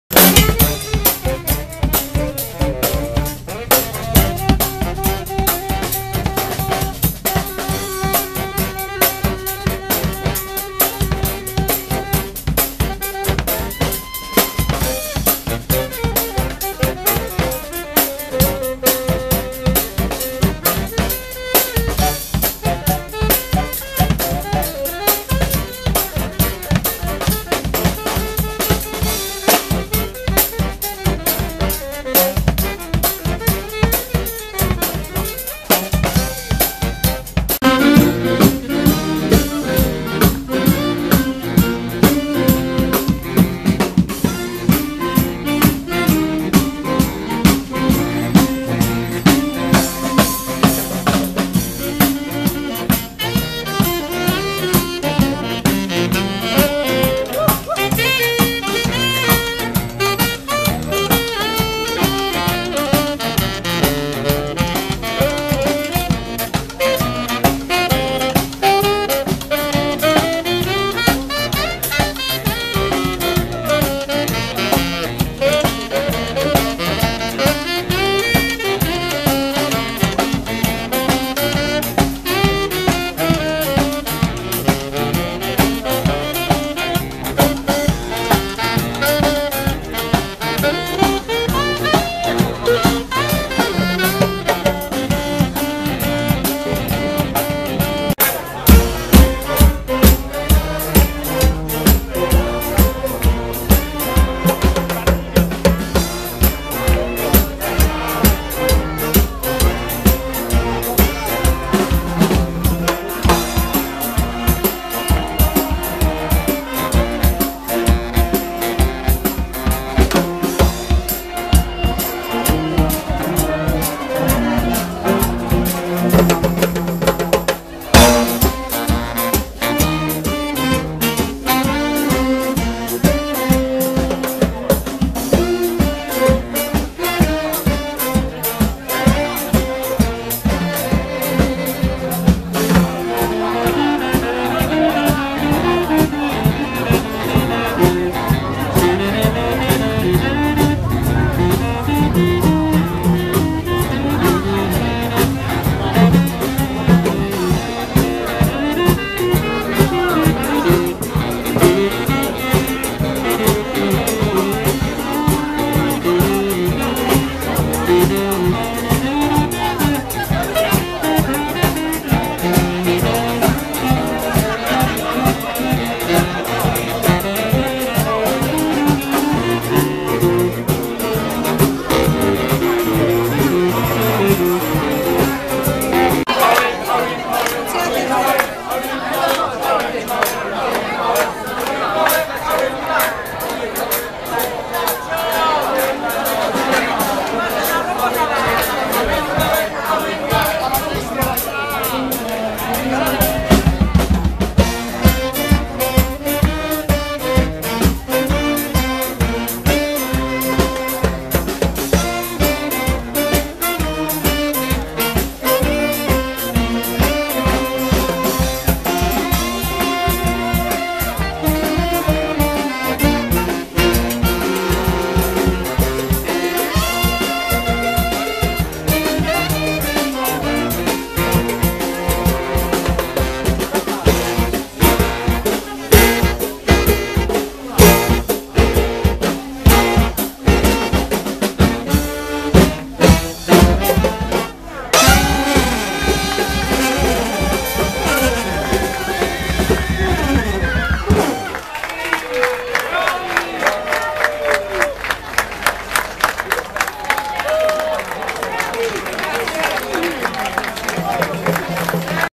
Telefunk'n @ Morgana & P.zza Della Vittoria - Formia
Dopo un'apparizione nelle vie del centro cittadino, durante la serata al Morgana c'è stata un incursione musicale della Street Band romana TELEFUNK'n.